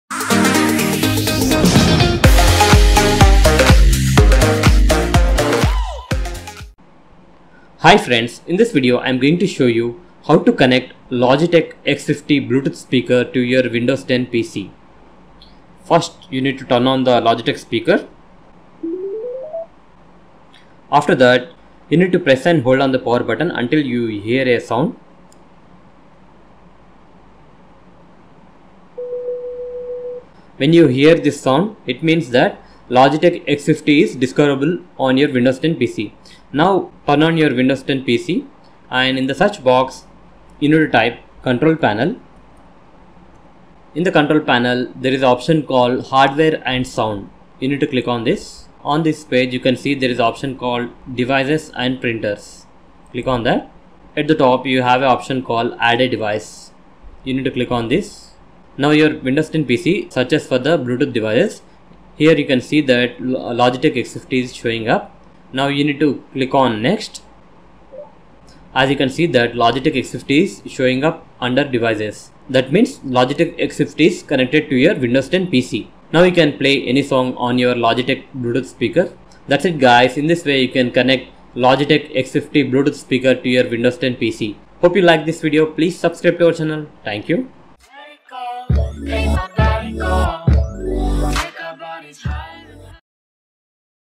How to connect (or) pair Logitech X50 Bluetooth Speaker with Windows 10 PC
in this video you will see How to connect or pair Logitech X50 Bluetooth Speaker with Windows 10 PC.